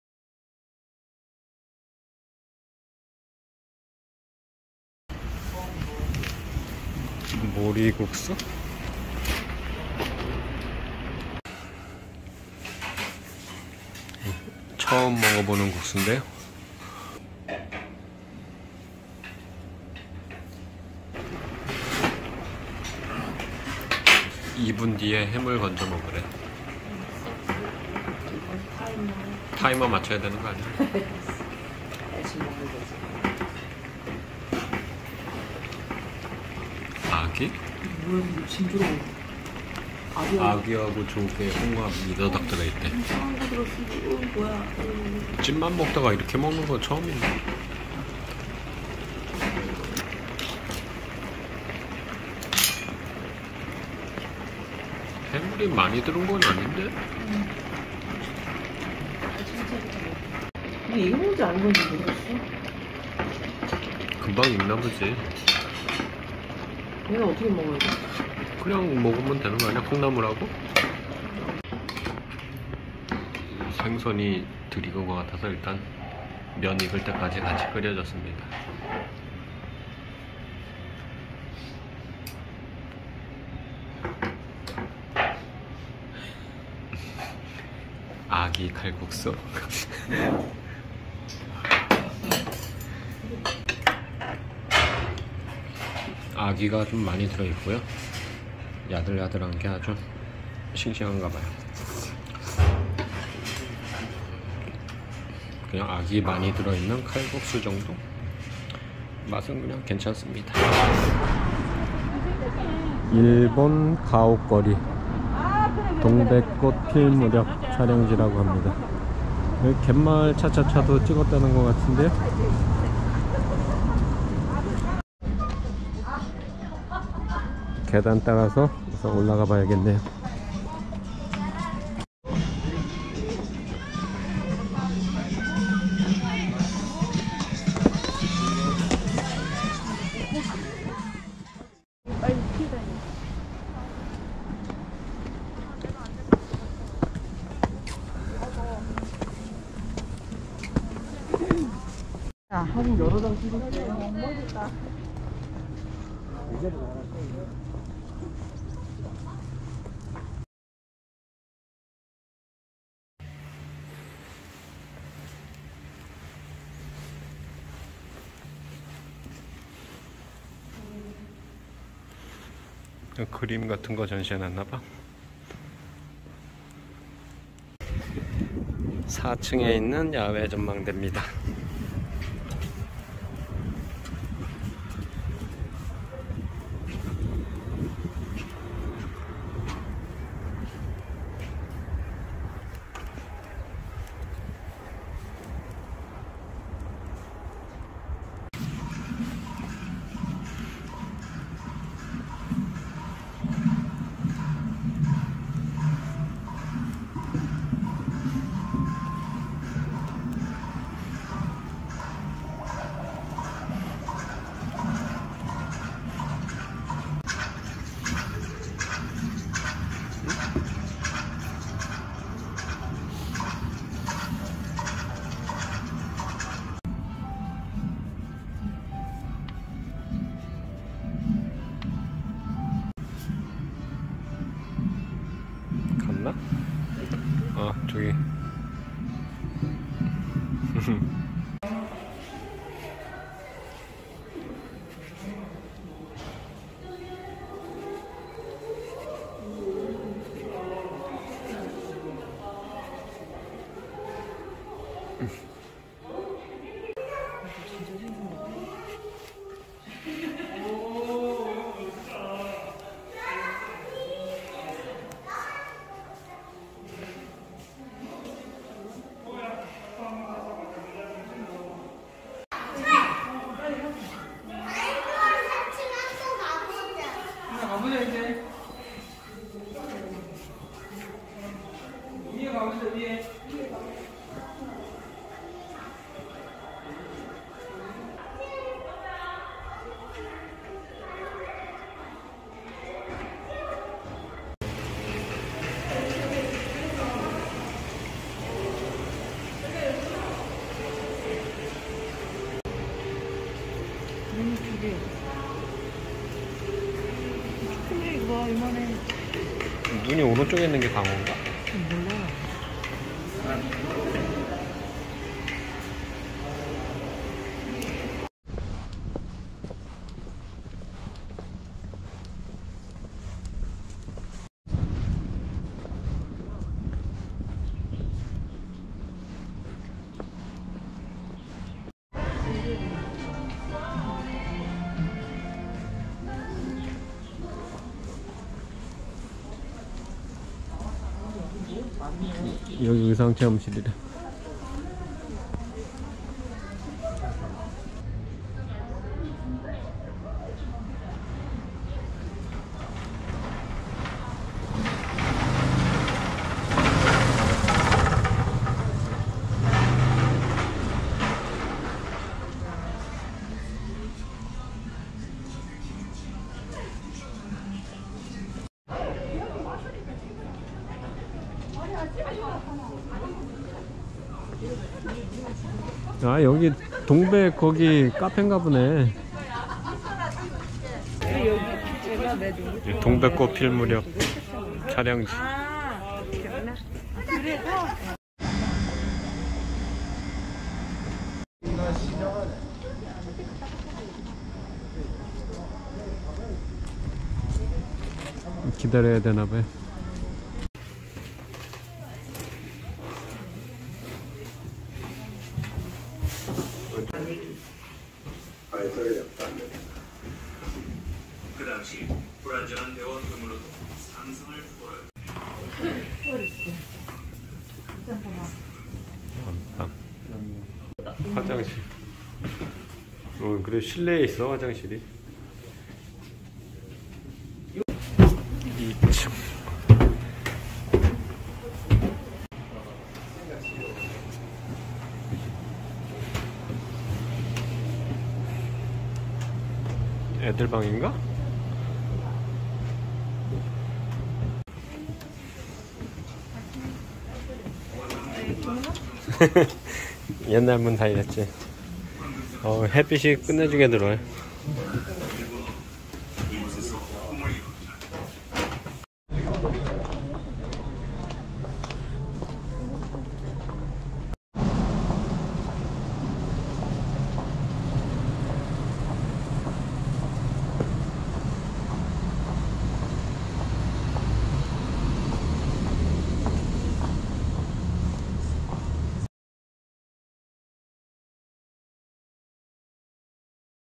포항여행 - 구룡포 일본가옥거리,과메기박물관(동백꽃 필무렵 촬영지)
포항 구룡포에 있는 일본가옥거리 입니다.
포항 모리국수 먹고 둘러보고 왔습니다.
과메기 박물관도 꼭 들려보세요.